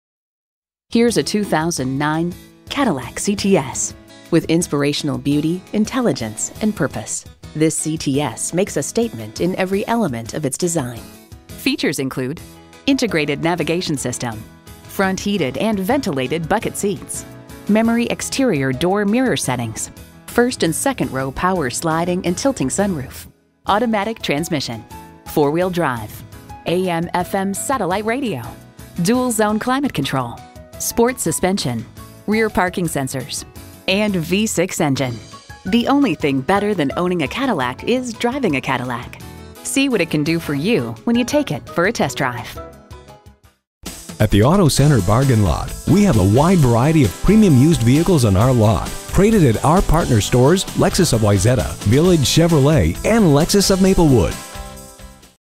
2009 Cadillac CTS Wayzata MN Minneapolis, MN
Year: 2009
Make: Cadillac
Model: CTS
Trim: Base
Engine: 3.6L V6 DI VVT
Transmission: 6-Speed Automatic
Color: Black
Interior: ebony
Mileage: 133040
Stock #: C26782B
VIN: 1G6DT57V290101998

AUTO CENTER BARGAIN LOT
1805 EAST WAYZATA BLVD,

DEPARTMENT HOURS
Sales
Monday - Thursday: 8:30am - 8:00pm
Friday - Saturday: 8:30am - 6:00pm